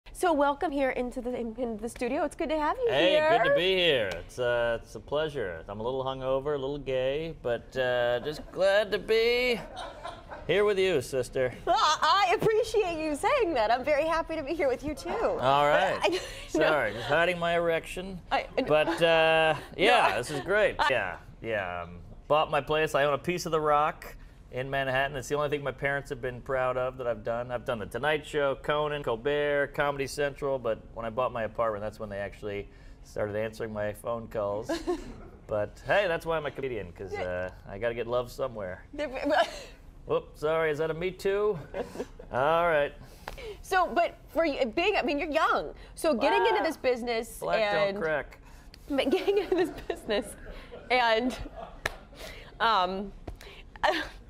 She was not ready for him...😂
Classic moment when @marknormand went on a LIVE Morning TV Show